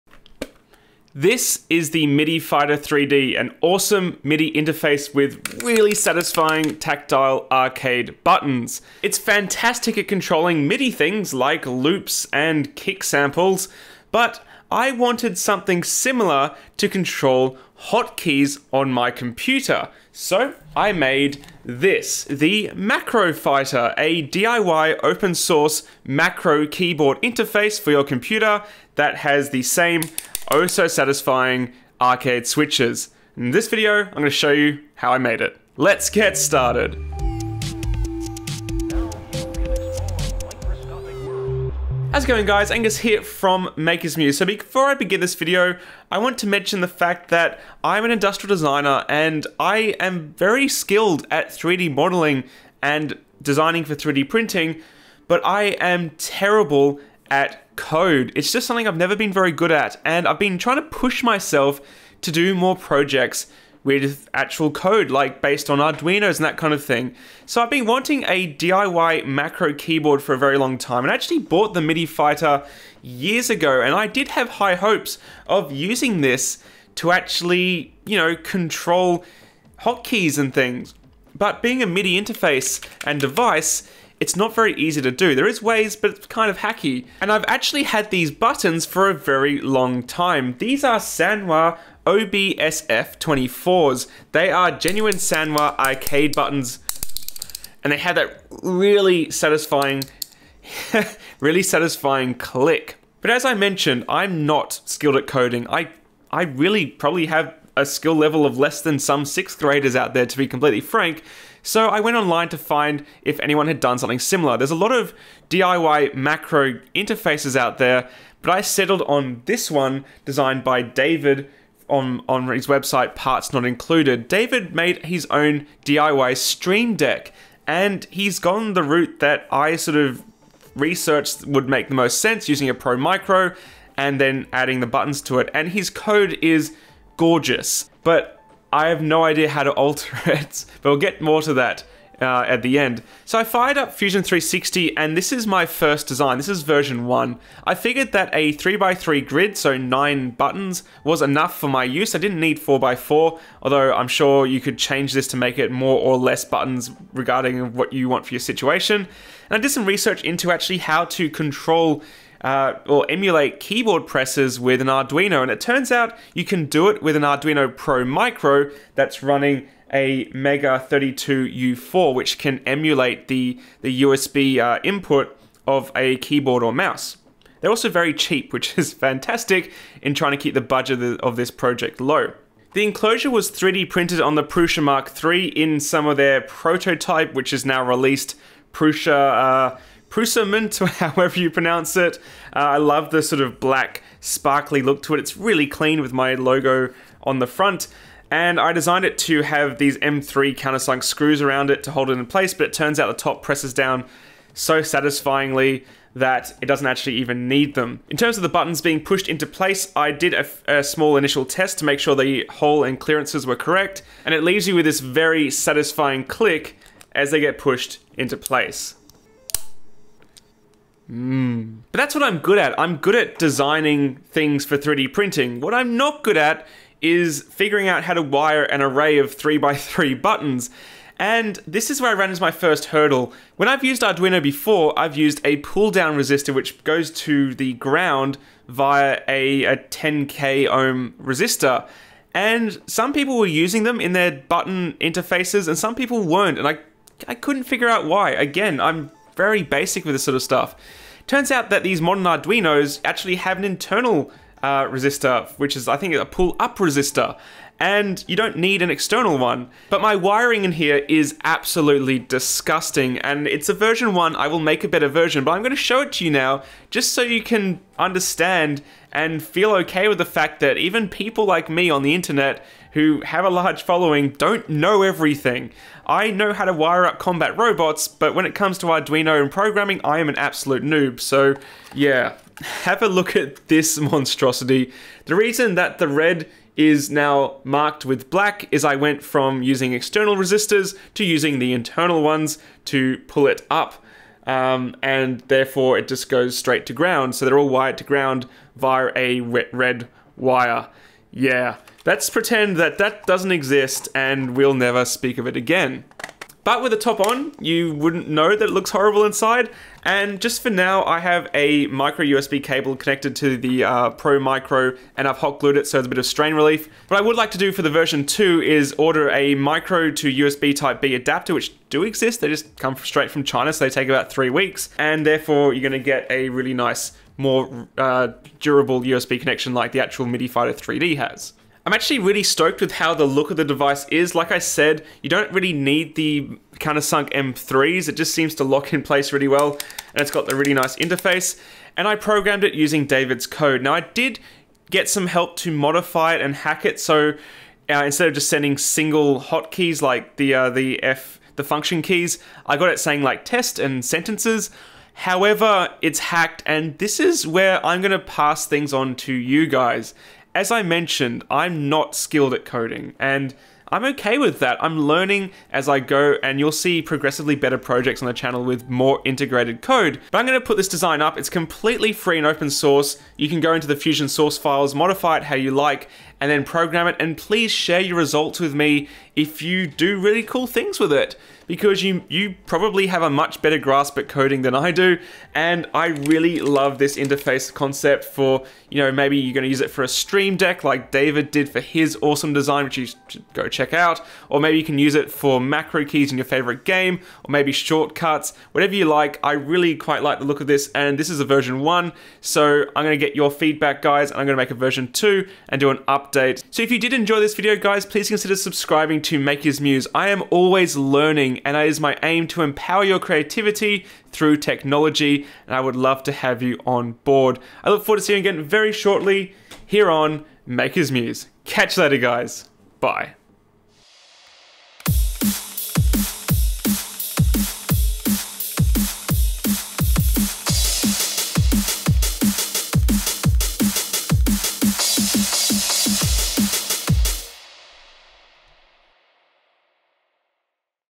Making a custom macro keyboard with REAL arcade buttons
This macro keyboard can be programmed to control hotkeys, type words and even execute shortcuts! It uses satisfying Sanwa OSBF-24 Arcade Pushbuttons in a 3DPrinted case.

For now, the files are available on Gumroad (enter 0 to download for free)

Programming is up to you! Be sure to check out David's awesome build of his DIY Stream Deck "Stream Cheap" for which I drew a lot of inspiration for this project.

3D Printing Essentials